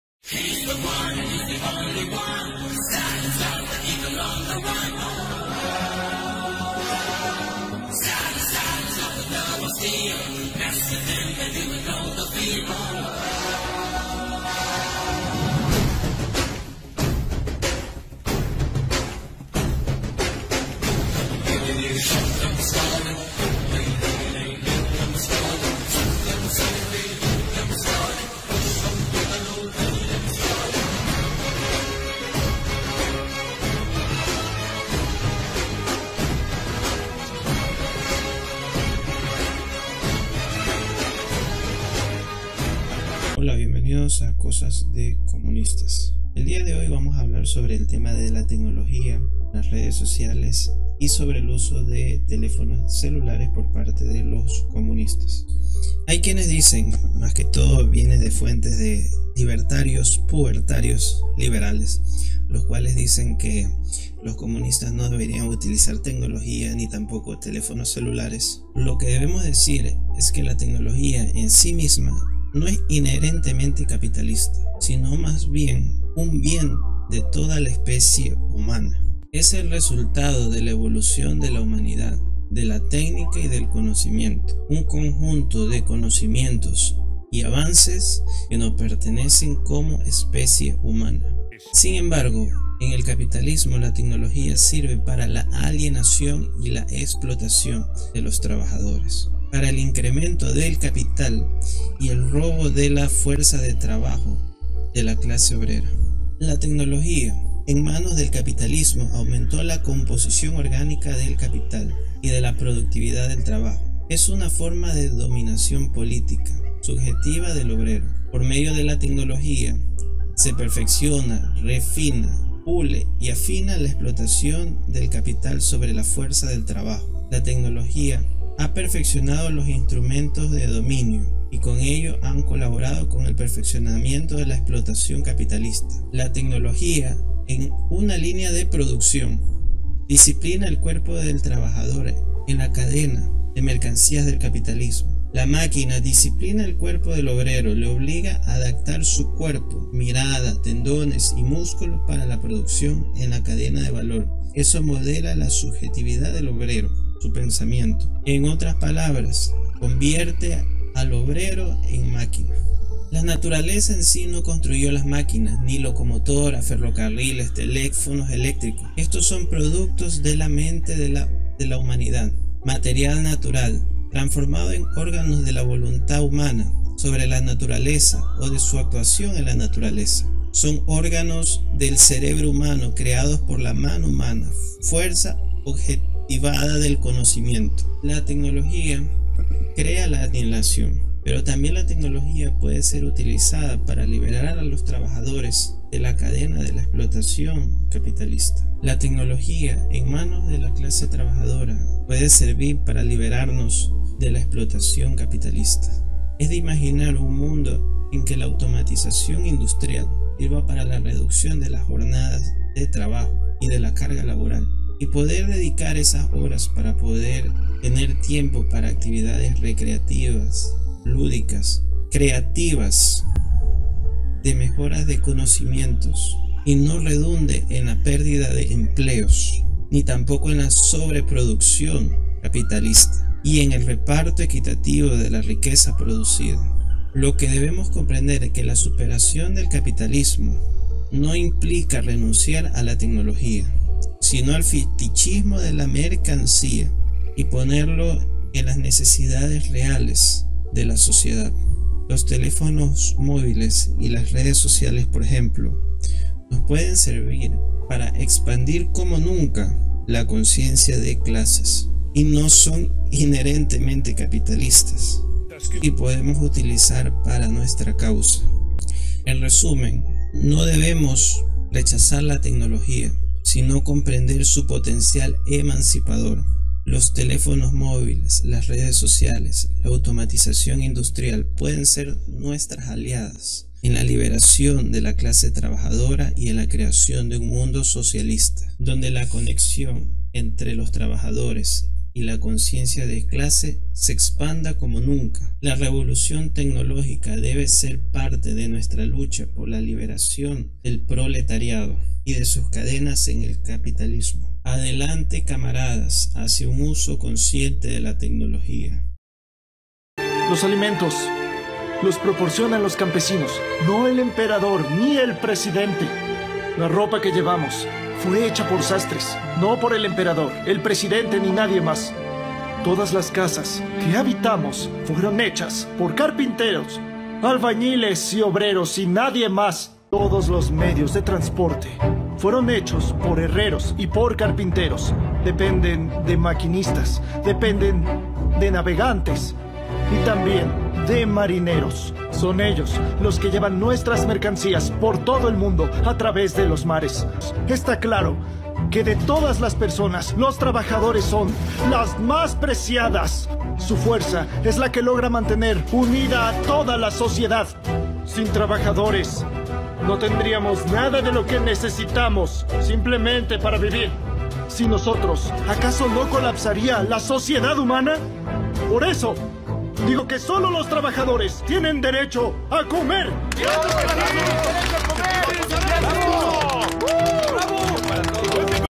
La TECNOLOGÍA y su POTENCIAL REVOLUCIONARIO
La tecnología e inteligencia artificial puede ser utilizada tanto para la alienación como para la liberación de la clase trabajadora en el capitalismo, pero en la lucha por la liberación se debe entender su potencial emancipador y usarla de manera consciente hacia un mundo socialista.